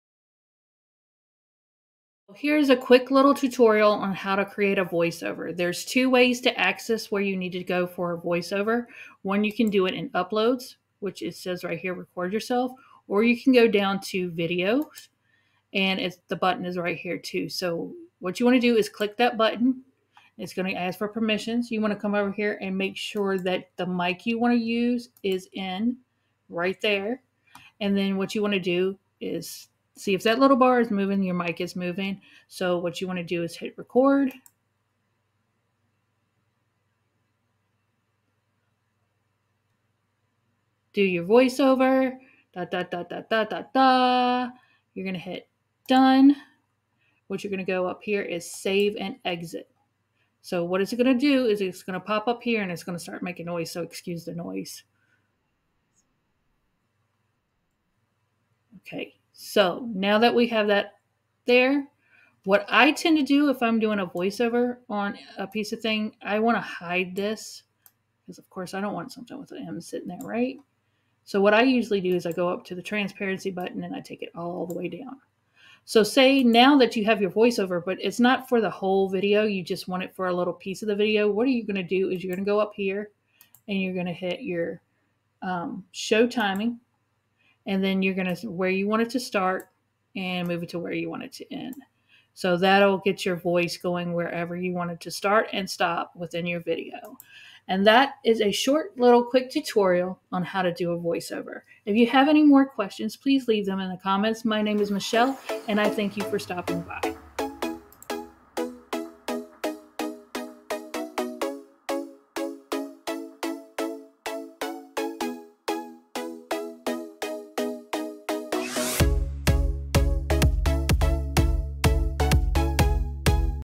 Canva Video Editing: How to Create Voice Overs in Canva
This tutorial will help you learn how to create voice overs for your videos in Canva. It will show you where the find the option to create a voice over, how to record the voice over and how to put that voice over with in your existing video. It will also show you how to time your voice over if you want to start  and end it in a particular part of your video. This tutorial will show you how easy it is to do.

All videos are 100% made in Canva